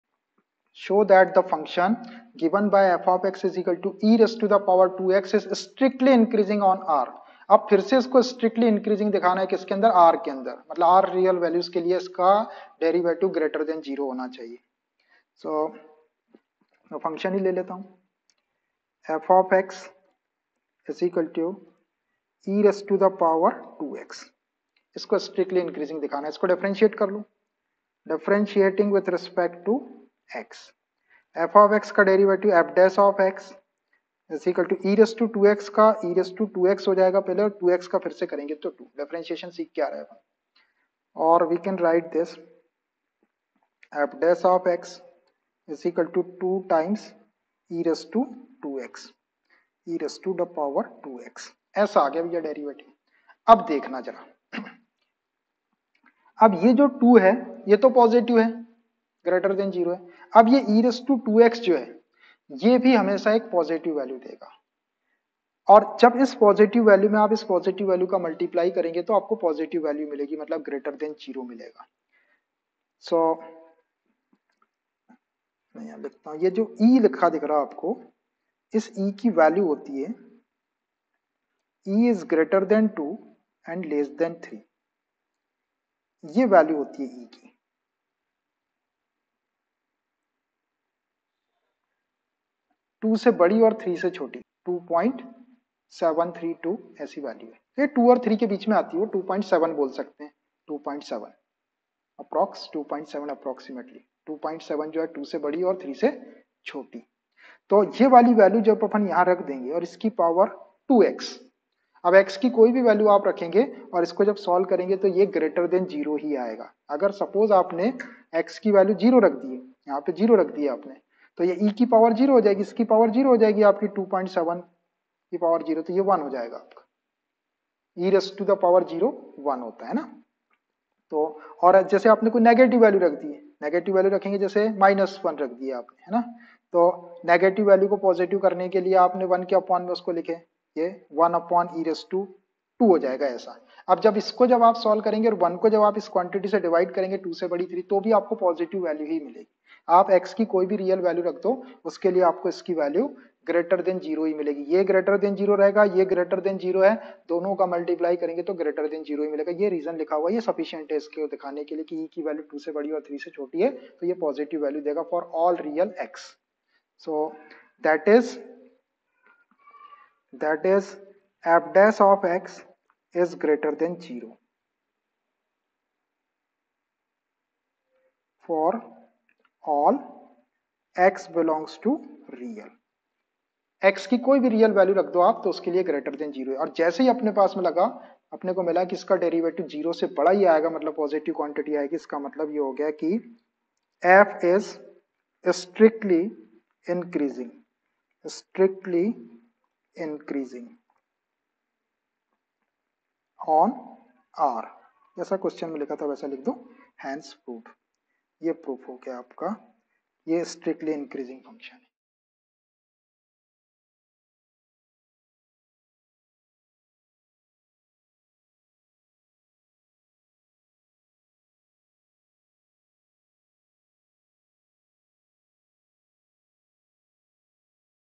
Application Of Derivatives Exercise 6.2 Q. No. 2 Ch 6 12th (NCERT)
Daud Sir Online Class

Class 12th (NCERT) Application Of Derivatives, Exercise - 6.2, Q. No. 2

2. Show that the function given by f (x) = e2x is strictly increasing on R.

NCERT & RD Sharma Videos for classes 12th, 11th, 10th & 9th